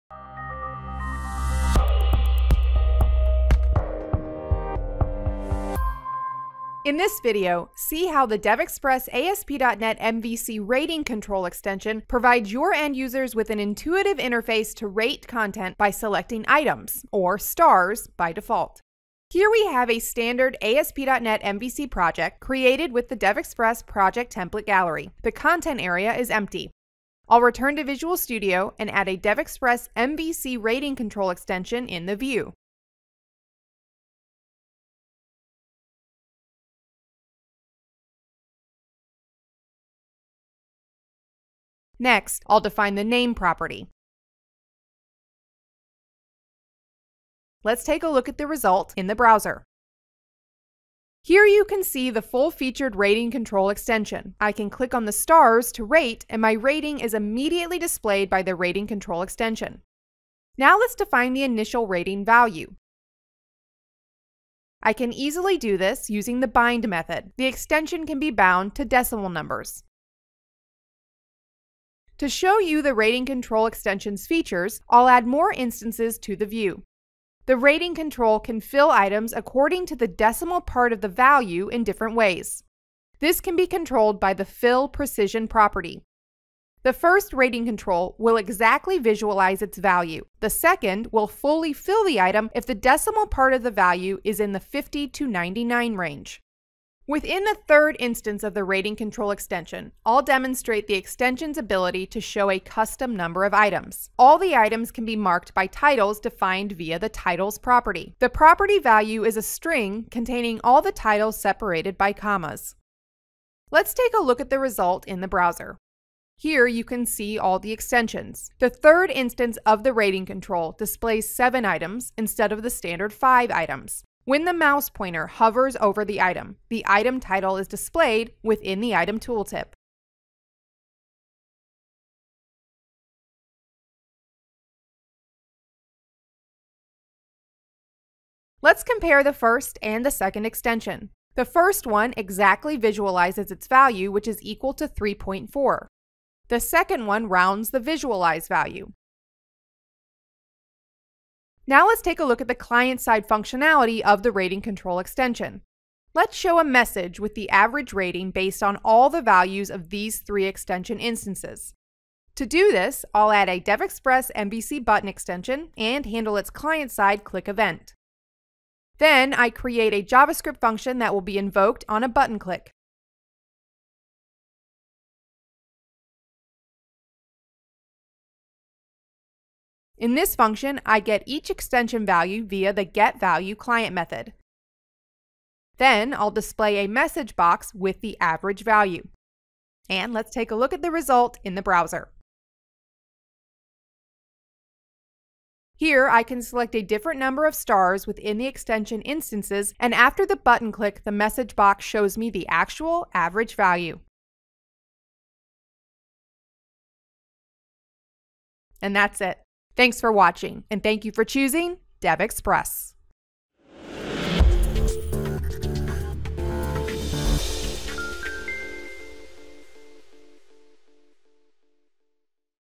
DevExpress ASP.NET MVC: Rating Extension
The DevExpress ASP.NET MVC Rating extension allows users to rate website content by clicking icons that represent ratings ("stars" by default). In this video you'll learn how to add a Rating extension to your web app and learn how to use its client-side API.